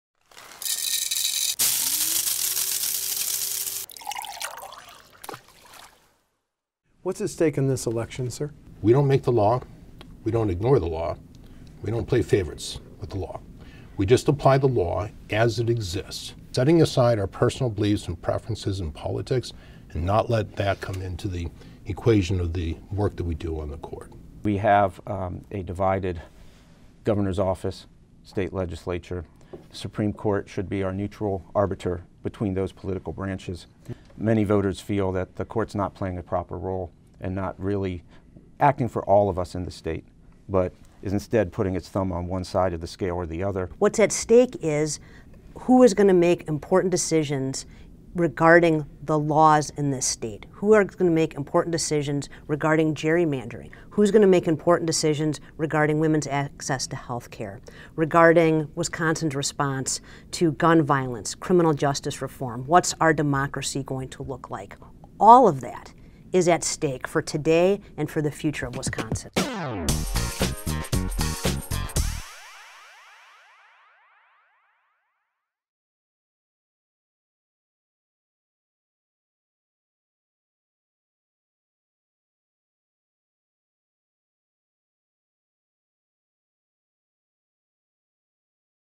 WisEye Morning Minute: Supreme Court Candidates
With the primary election on February 18, WisconsinEye is featuring the Wisconsin Supreme Court candidates Jill Karofsky, Justice Daniel Kelly, and Ed Fallone responding to the pressing question of "what's at stake in this election?"